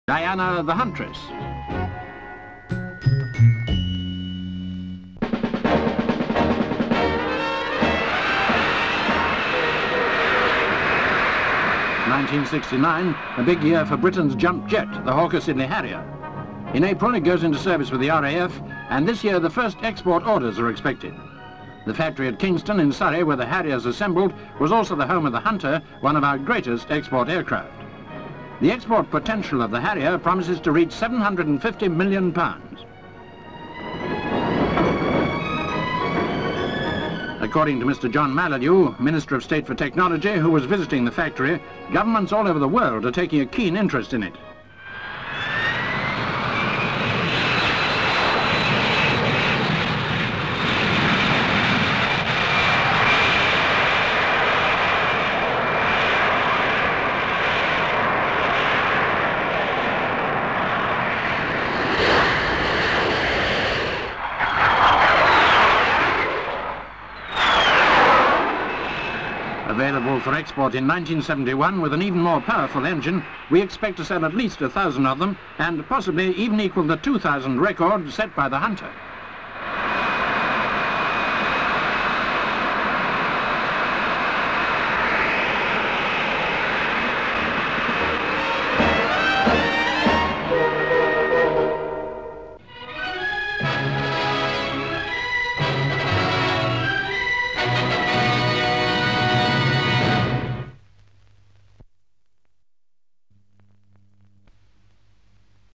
EXPORTS - STRAIGHT UP
(10 Mar 1969) Some spectacular flying by Britain's Jump Jet, the Harrier, on show at Dunsfold after a visit to the factory where this revolutionary plane is built. Export expectations could realise seven hundred and fifty million pounds, so great is the overseas interest in this incredible machine.